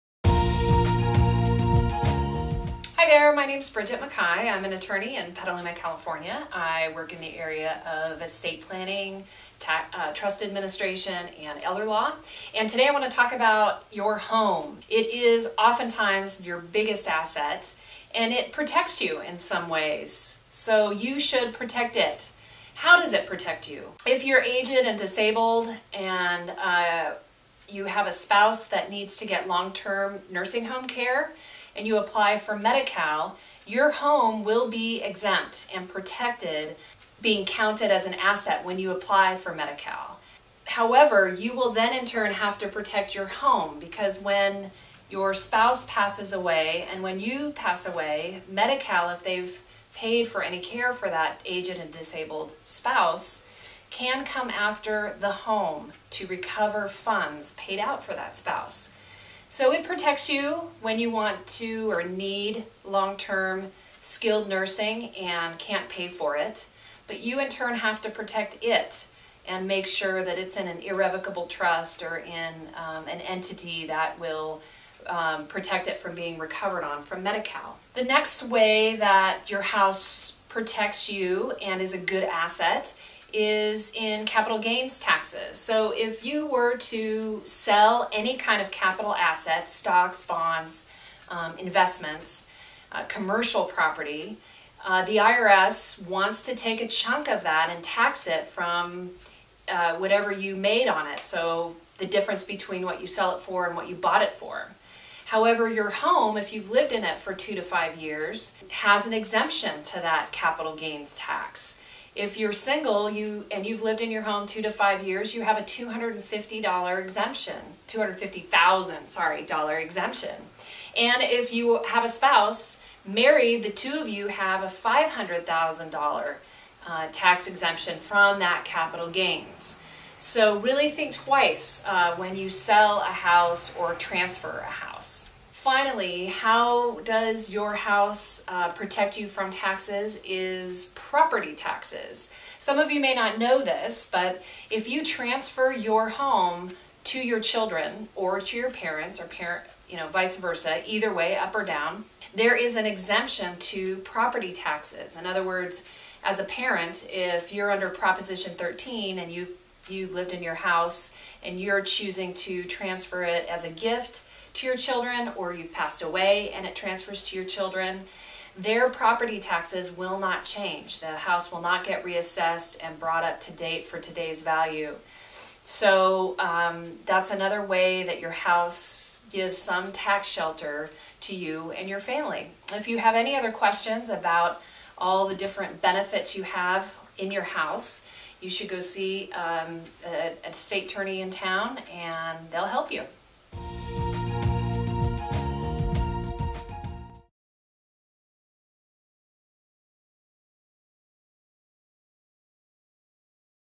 Fund Long Term Care For Elders With a House
Serving Santa Rosa and Petaluma, estate planning attorney Bridget Mackay discusses how home owners can use their home as a tool to finance long term care services.